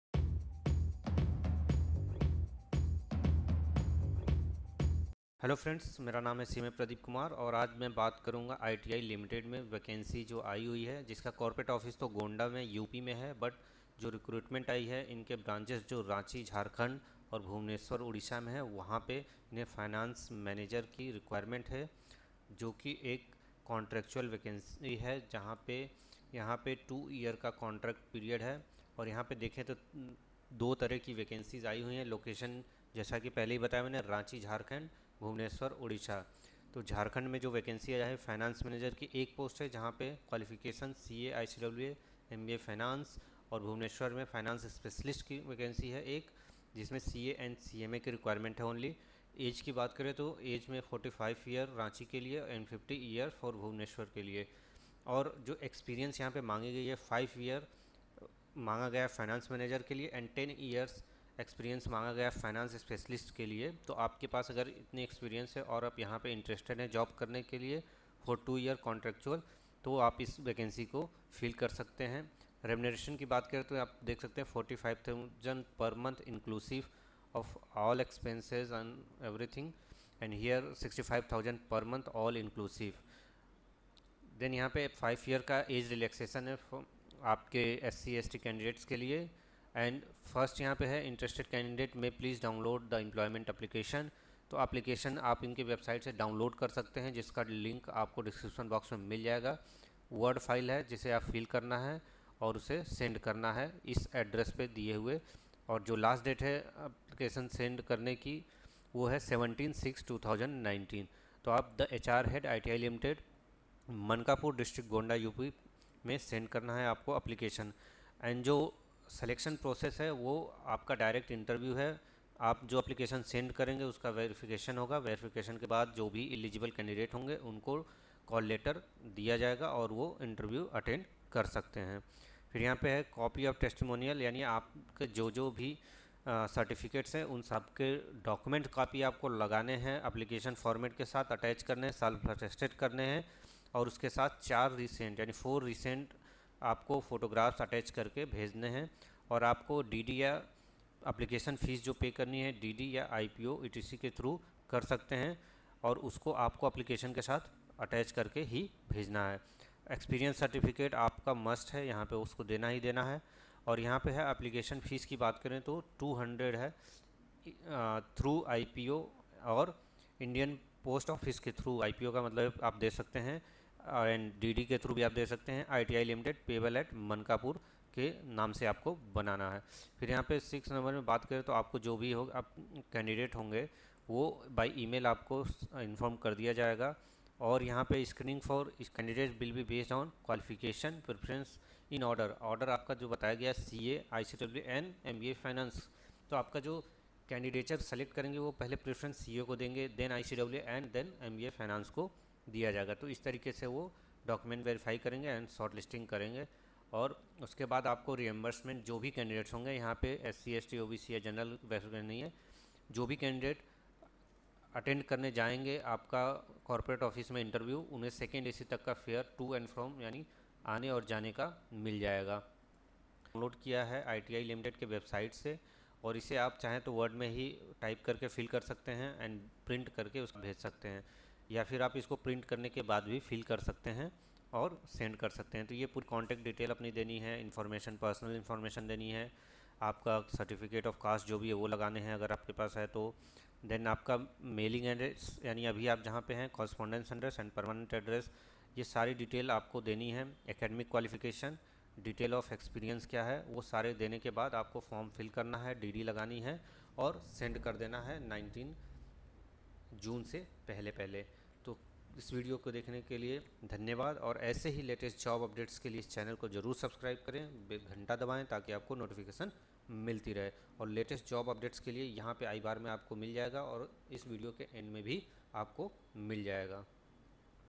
ITI Limited I FINANCE MANAGER/ FINANCE SPECIALIST I Experience job
ITI Limited, Mankapur is looking for Finance Manager for Ranchi (Jharkhand) & Finance Specialist for Bhubaneshwar (Odisha) for TPA (Third Party Audit) Project on contract basis for a period of two years.

ITILimitedRecruitment2019